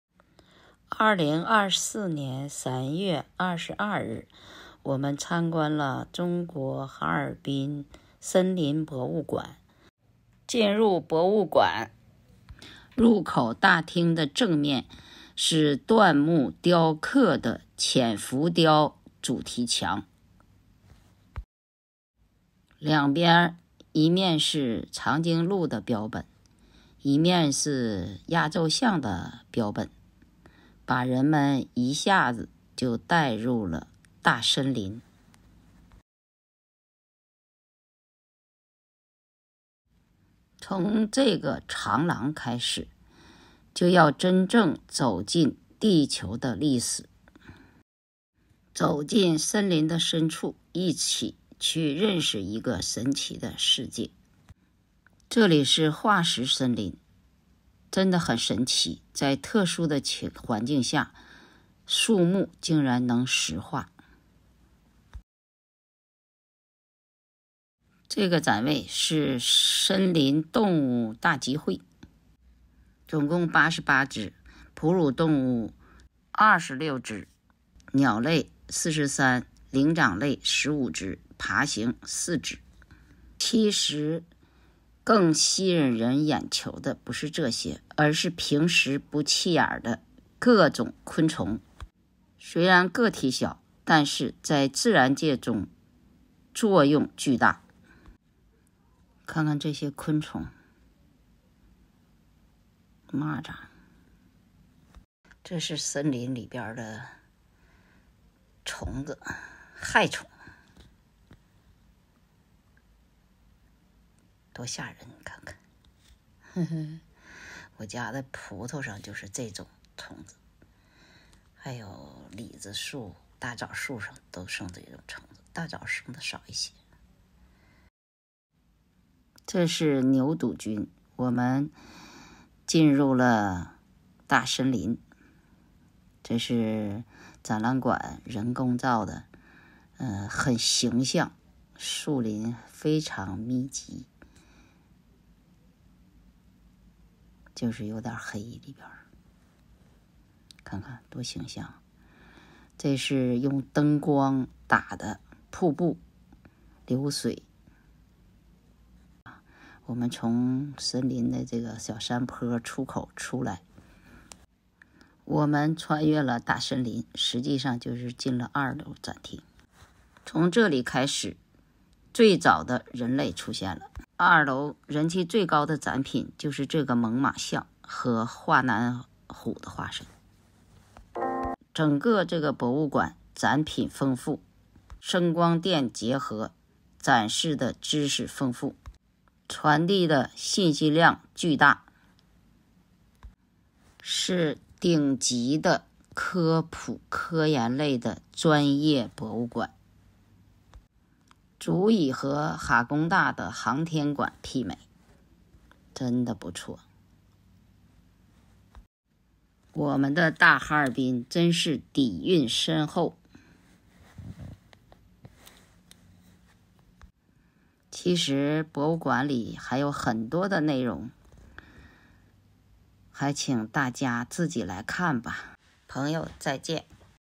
中国（哈尔滨）森林博物馆纪实
2024年3月22日参观中国（哈尔滨）森林博物馆纪实。